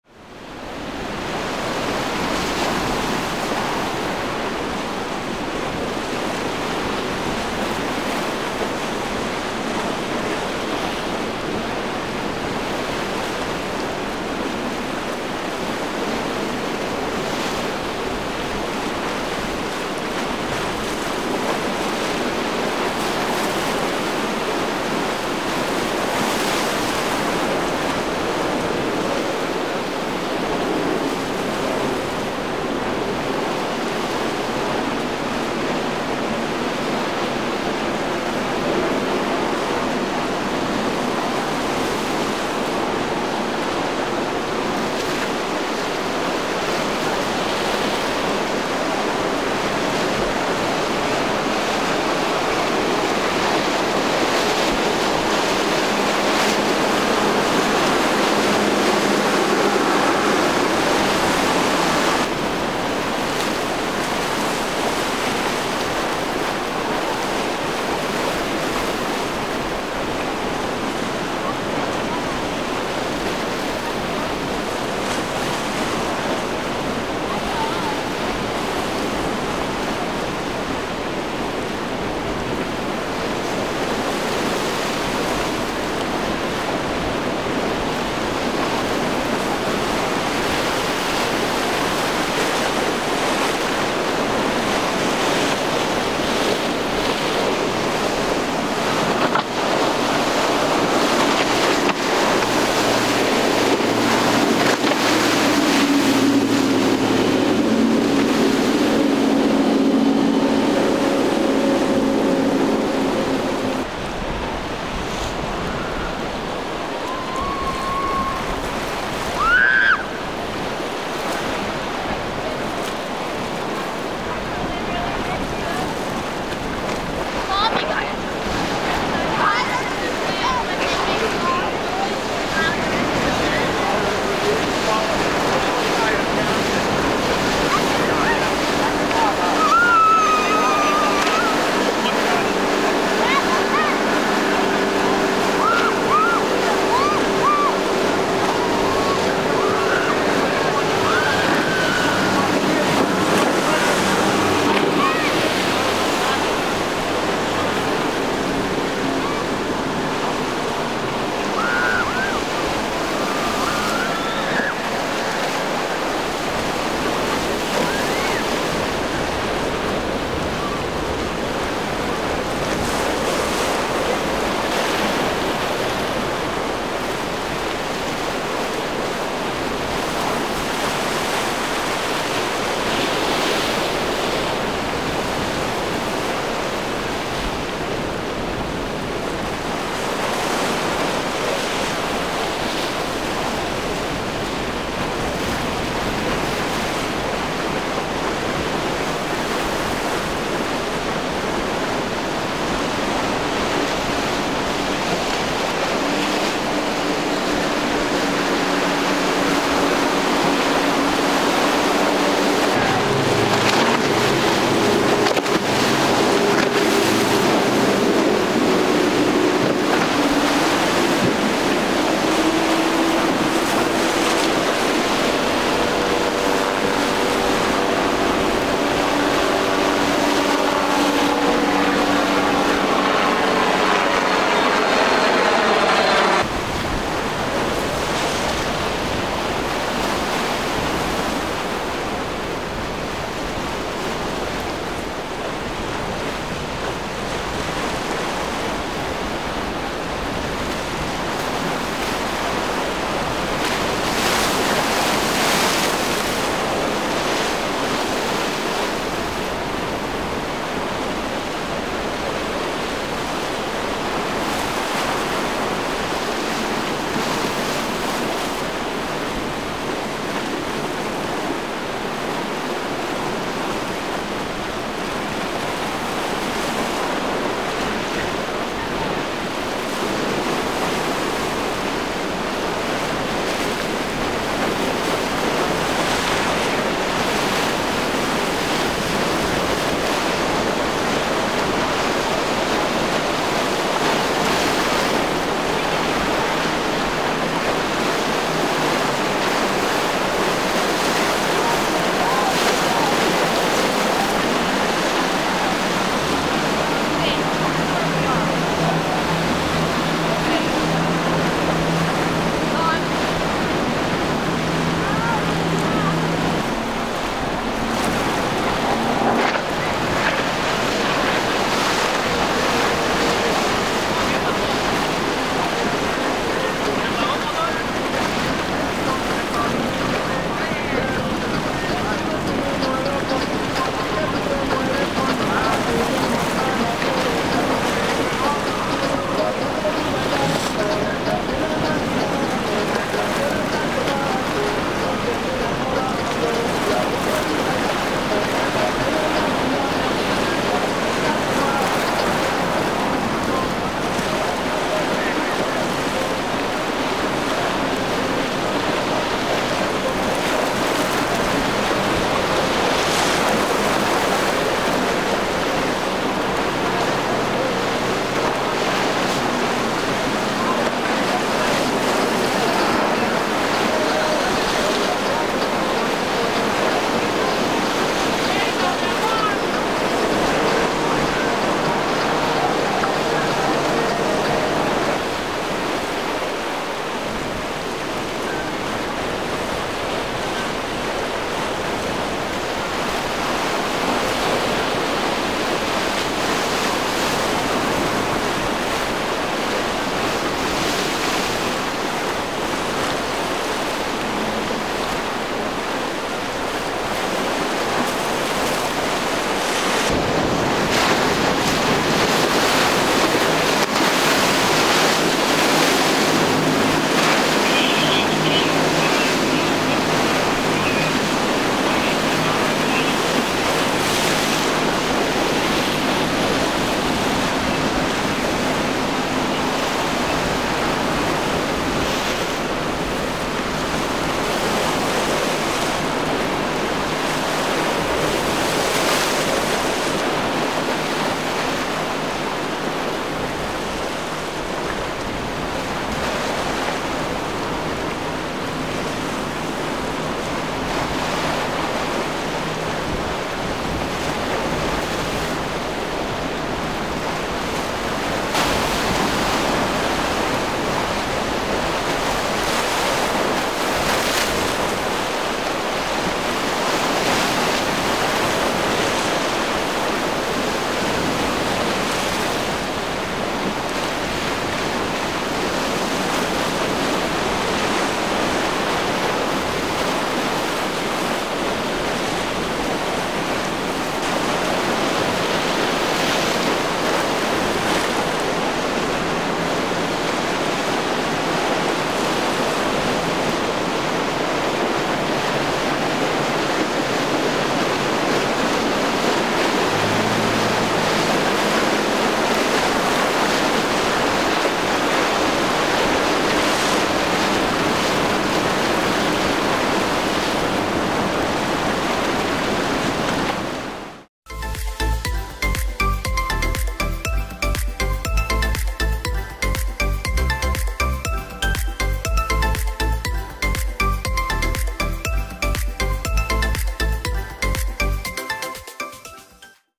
BOAT LIFE: HOT DAY AT BOCA INLET
Epic day at Boca Inlet with great boating weather condition. Plenty of actions and ton of great boats!
As always, thank you for visiting my channel! Lot more great content on the way!

This video was created for educational and entertaining purpose.